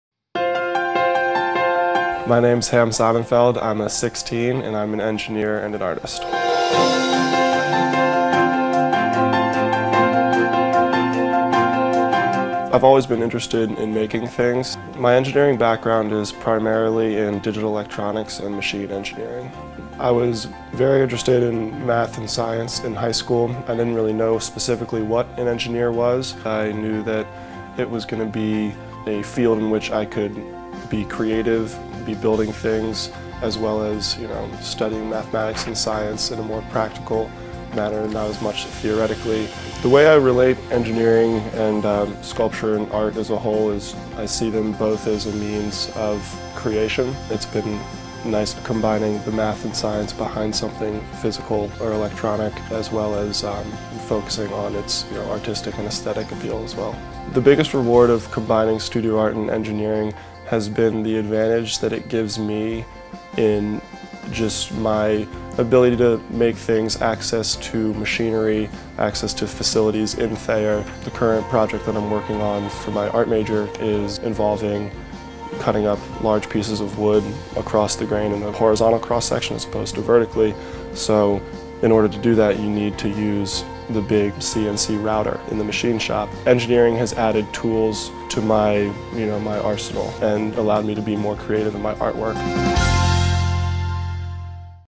I'm an Engineer and an Artist
Ham Sonnenfeld '16 discusses combining engineering with studio art at Dartmouth.

Video edited by Rusty Spydell.